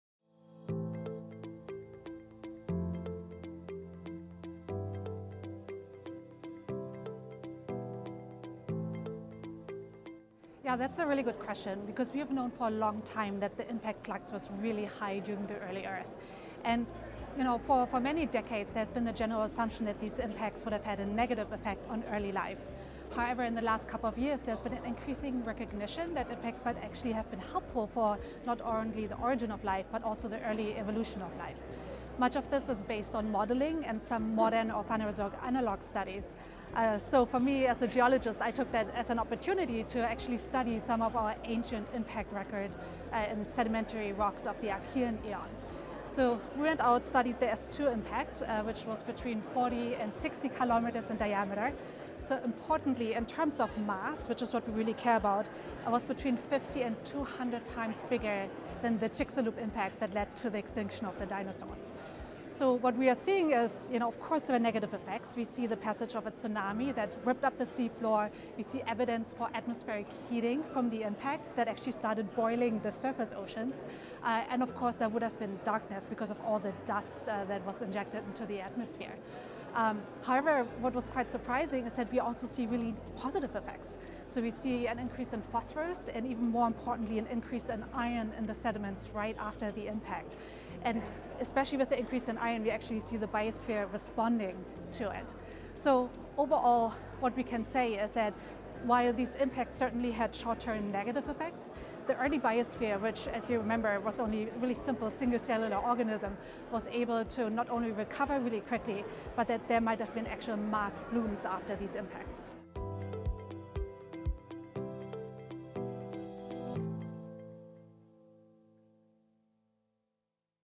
☄️ When Giants Struck: Nadja Drabon on Meteor Impacts & Early Earth
Dr. Nadja Drabon of Harvard University brings early Earth to life with her research on giant meteor impacts and the clues they leave behind in ancient rocks. In this GSA Connectscast feature, she explores how these cataclysmic events shaped our planet’s crust, atmosphere, and potential for life.

🎥 Watch to discover how one of Earth’s most violent eras paved the way for everything that followed.